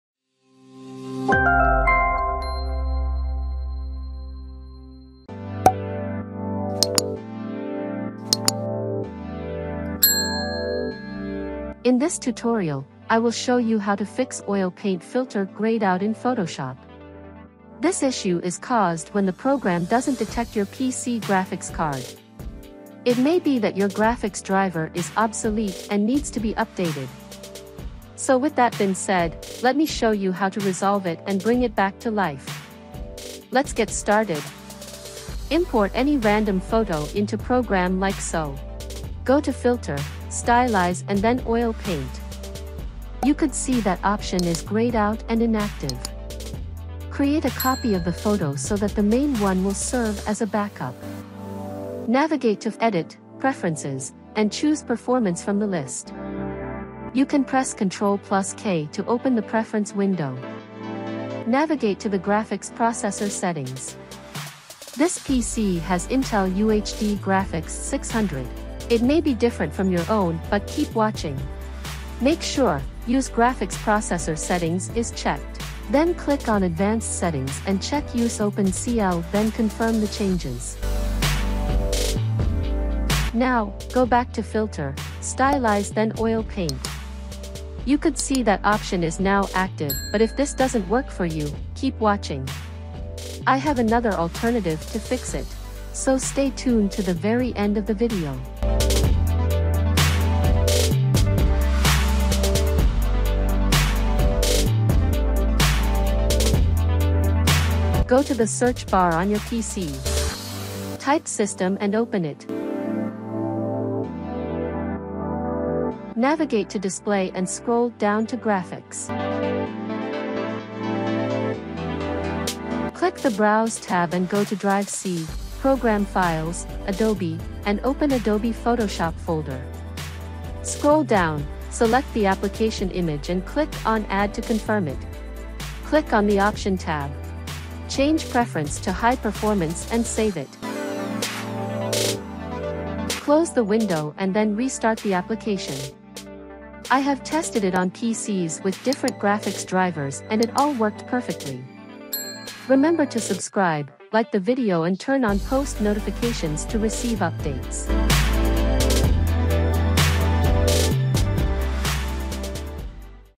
Let's Fix Oil Paint Filter Grayed Out & Inactive in Photoshop | Working 💯
In this tutorial, I will show you how to fix the oil paint filter grayed out and inactive in photoshop.